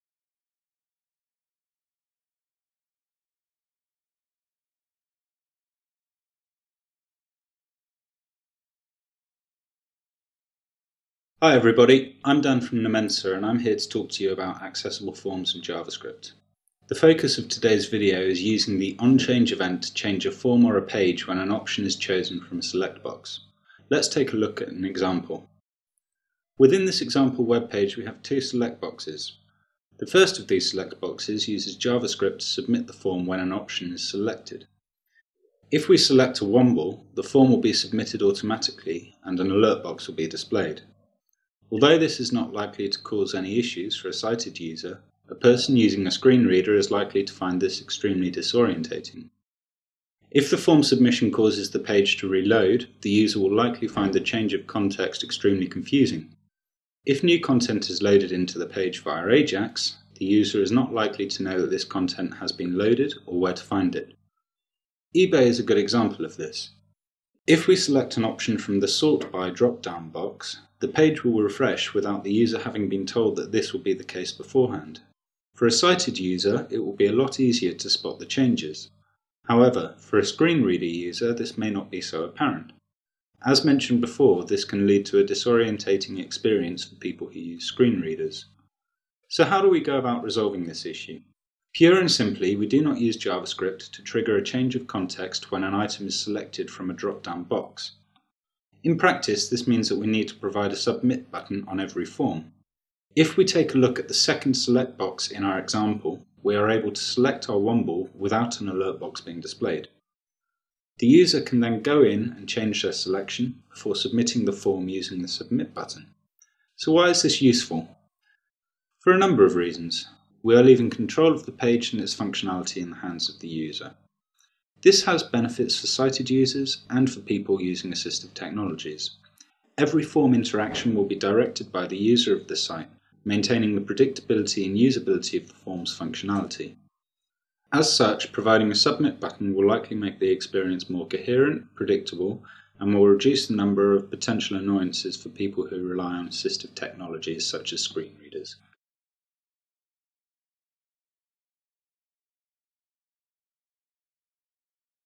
Accessible Forms & Javascript
The first in a series of How To guides from Nomensa. Dan Stringer, Web Developer at Nomensa discusses what to do and importantly what not to do when using Javascript and forms. As part of this tutorial, Dan provides solutions to common mistakes.